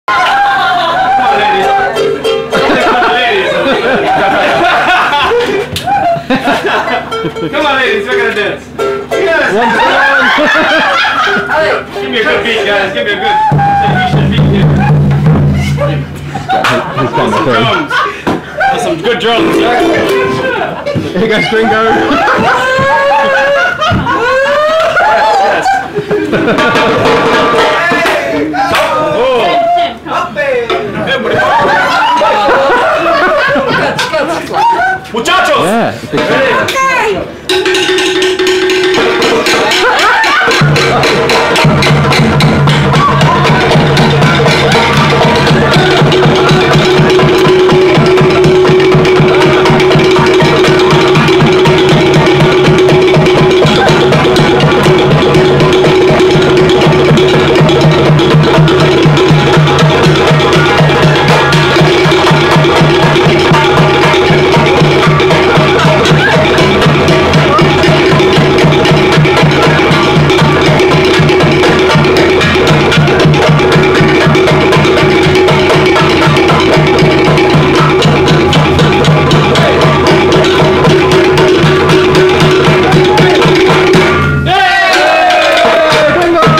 The traditional Tahitian "Gringo" dance
Gringo With A Green Bag busts some killer moves aboard the Haumana Cruise in Bora Bora, Tahiti - French Polynesia.

*No locals were harmed in the process...physically

"Dance like no one is watching...or like you had a mask on"